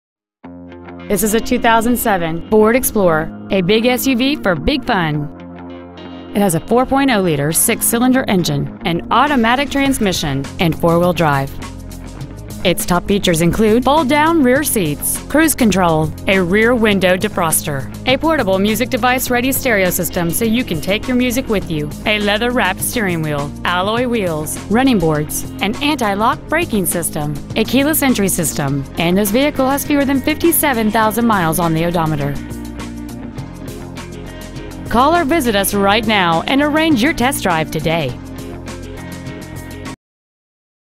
2007 Ford Explorer Arlington WA
Year: 2007
 Make: Ford
 Model: Explorer
 Engine: 4.0L V6 EFI
 Trans.: Automatic
 Exterior: White
 Miles: 56,534
 Interior: Gray

 16632 Smokey Point Blvd.

 2007 Ford Explorer Arlington WA